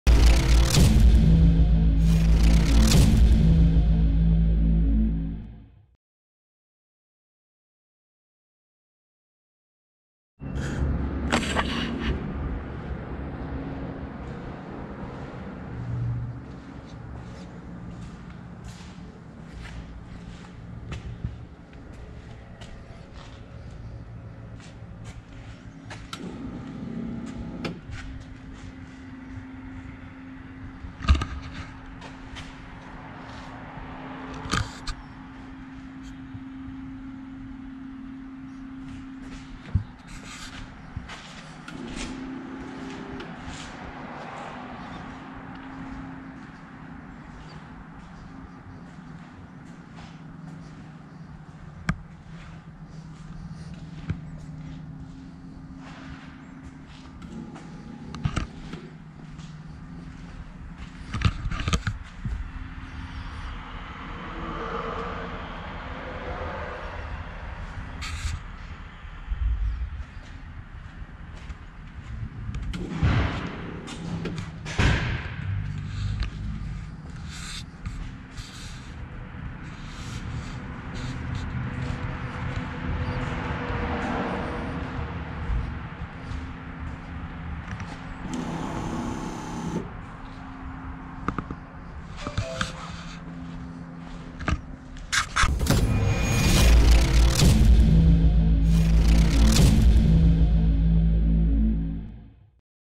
1993 Dodge Ram 250 For Sale - Walk Around Video (128K Miles)
1993 Dodge Ram 250 For Sale - Walk Around Video (50K Miles)
This 1993 Dodge Ram 250 walk around is brought to you by GR Auto Gallery, complete with underside of the Ram, as well as start up and running footage of the 93' Dodge Ram.